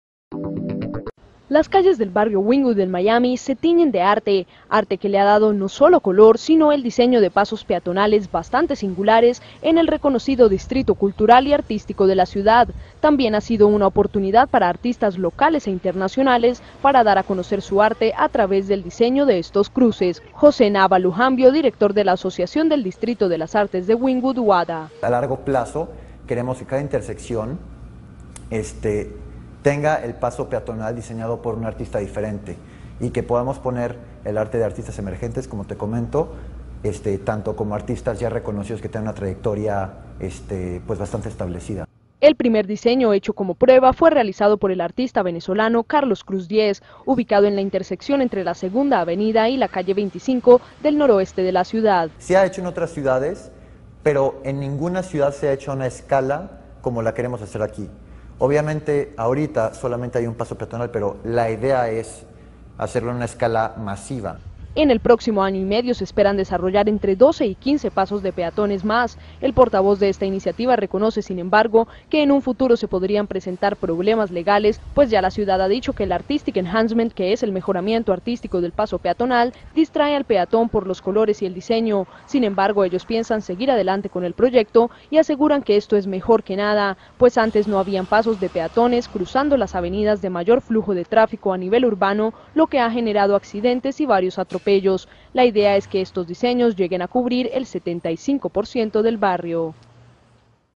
El arte de cruzar la calle
El barrio de Wynwood, reconocido distrito cultural y artístico de Miami, ha lanzado una iniciativa para renovar sus pasos de peatones y ofrecer una oportunidad a artistas locales e internacionales para dar a conocer su arte a través del diseño de estos cruces.